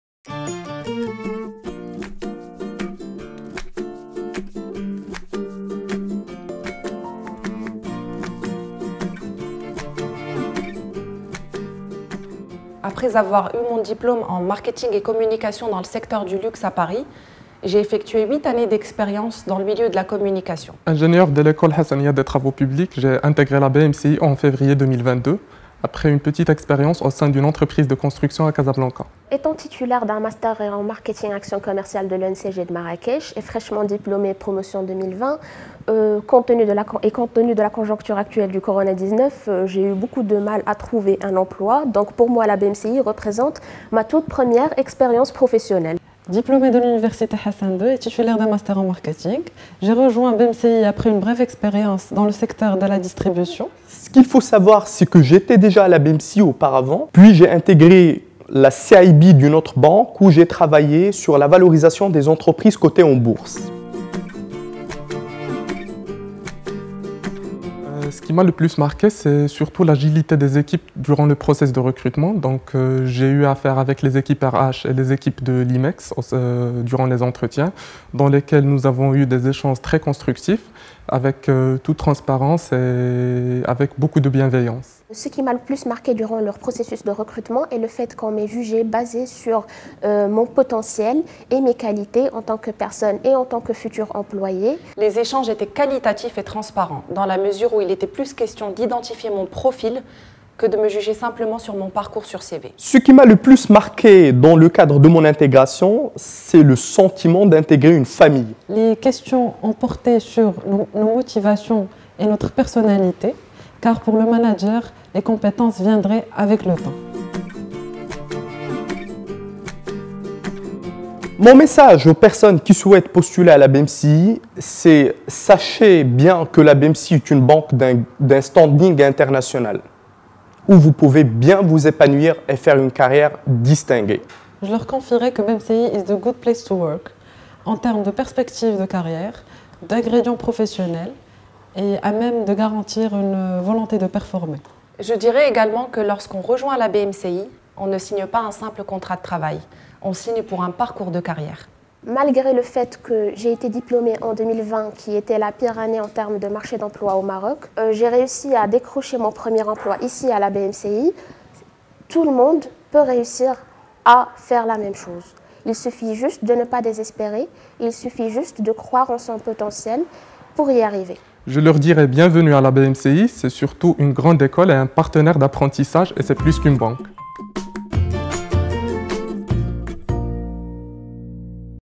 Nouvelles Recrues BMCI
Un Onboarding réussi !
Nos nouvelles recrues partagent leur parcours fluide de recrutement au sein de la BMCI :
Plus qu'une banque, la BMCI est un partenaire d'apprentissage !